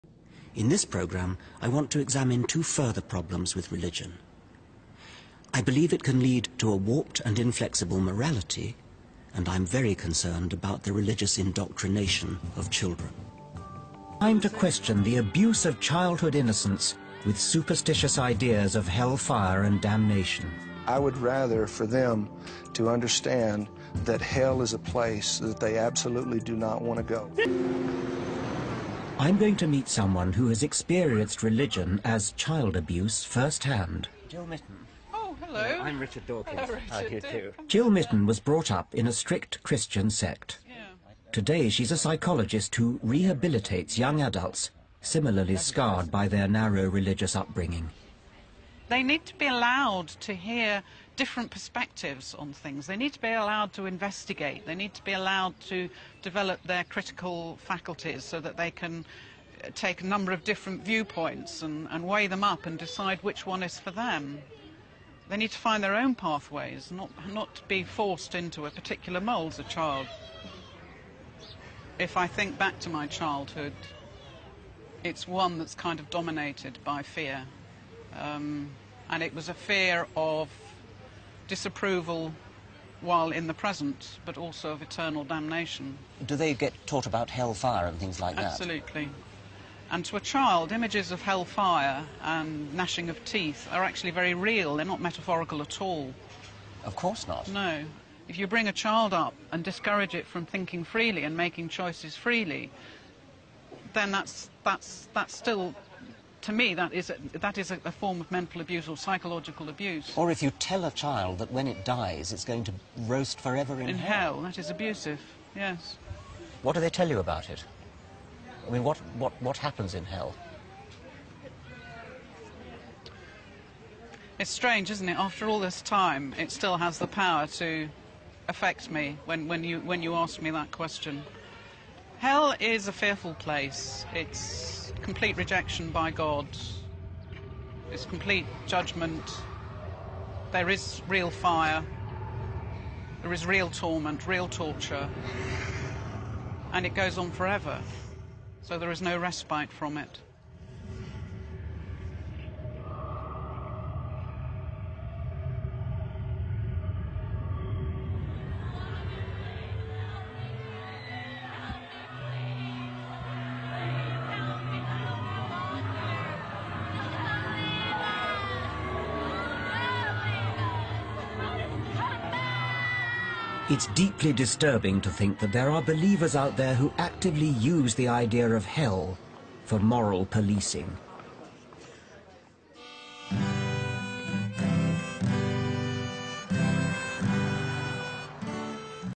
R Dawkins: Abusi sui bambini insegnando superstizioni come l'inferno di fuoco
Abusi sui bambini insegnando la falsa dottrina dell'inferno di fuoco
Intervista di R. Dawkins
Per sapere la verità sulla condizione dei morti, visitare JW.org
Digitando la parola "inferno", per esempio troverete:
"Cos’è l’inferno? Un luogo di tormento eterno?"